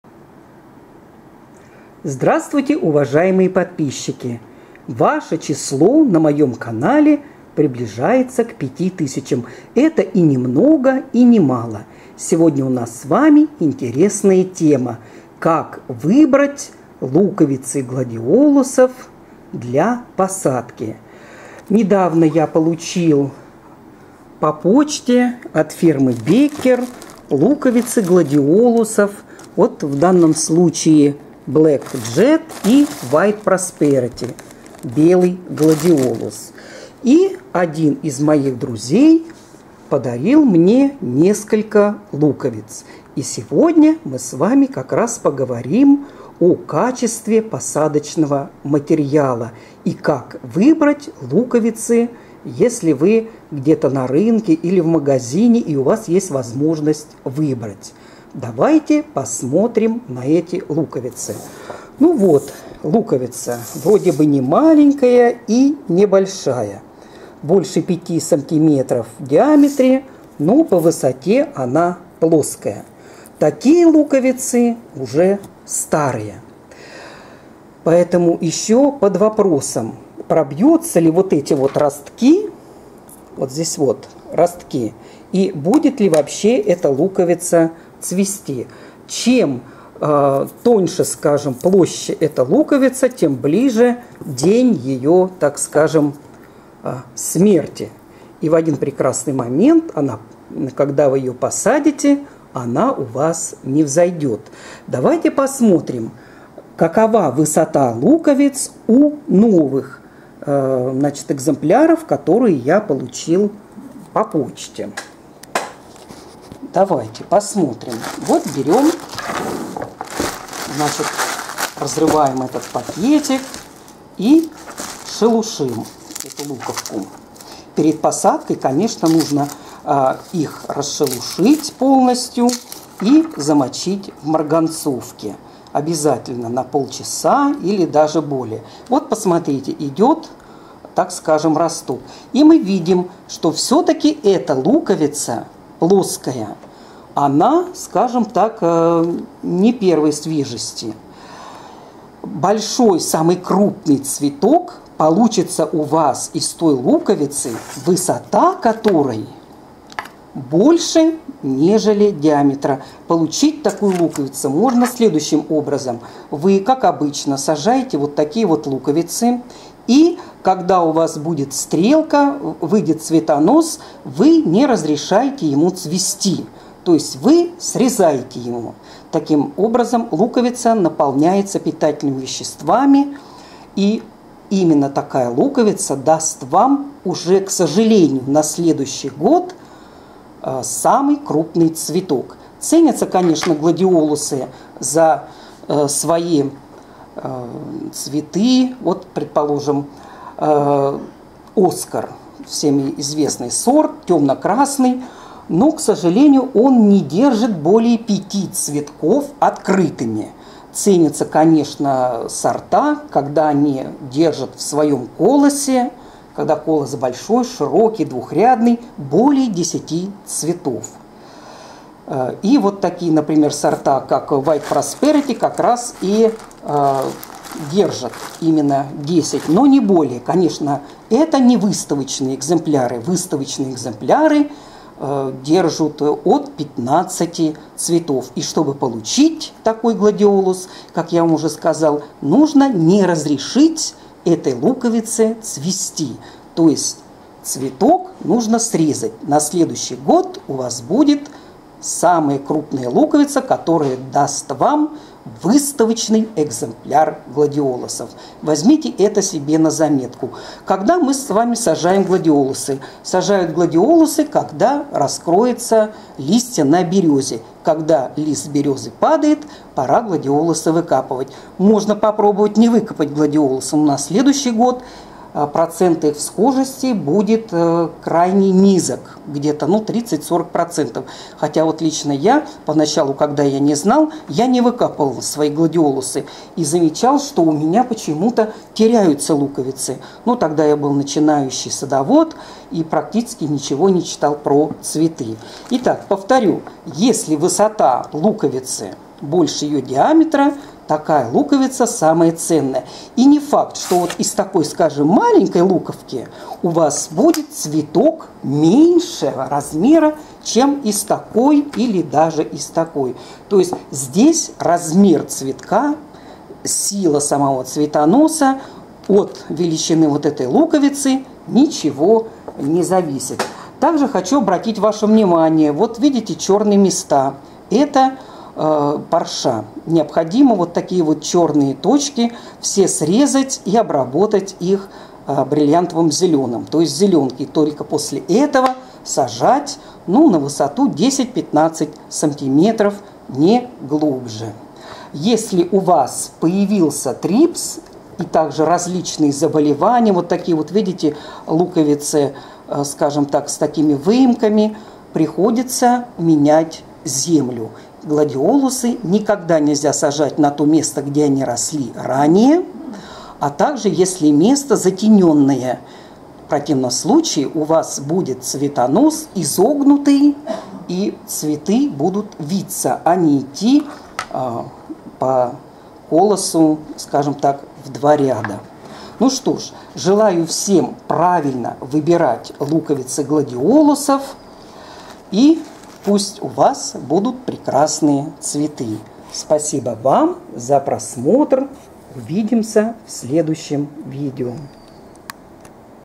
Как правильно выбрать луковицы гладиолусов?
В этом видео рассказываю о том, как правильно выбрать луковицы гладиолусов. Рассказываю небольшие секреты ухода